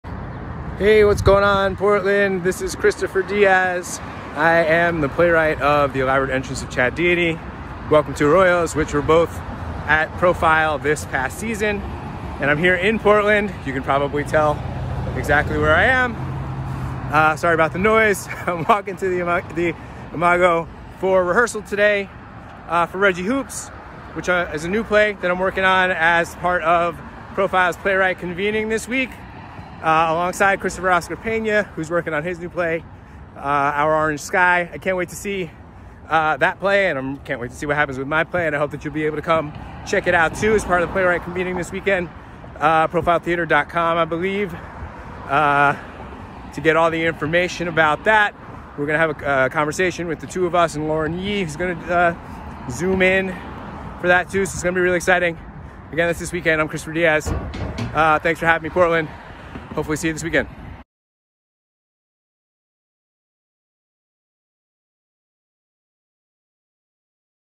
Kristoffer Diaz Convening1
Playwright Kristoffer Diaz chats about Profile Theatre's Playwright Convening, new works, and his first time in Portland.